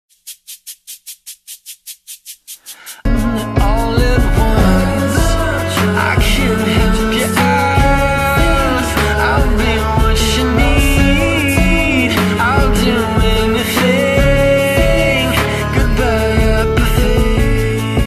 +c h a n g e
[HD makes things bearable.]
(this was that ipod shuffle epiphany song)

so much is changing, like I've been waiting and willing change for the last four months and it's all hitting me at once and I don't know how to handle it honestly.
I didn't think losing this pony would be so hard. But he was so much more than that. I've slowly been losing my most important constants in my life one-by-one.  He was my best friend, my babysitter, my confidence boost when I needed it. He was something solid for me to get on and ride. He was the one that was always the last to be ridden because it was a guaranteed good ride and now he's gone. And gone in this case, means gone for good. I'll never get a chance at him again because I can't ride him anymore, but he's missed so very terribly.
I avoided the barn today, simply because I know he's not there.
I'm so ready for my bracelet to be made from his tail. :\
goodbye apathy.
him leaving has completely ignited my will to ride again. I'm not bored and I'm ready for new things that are coming, but I miss him so freaking much it's unbearable right now.

-3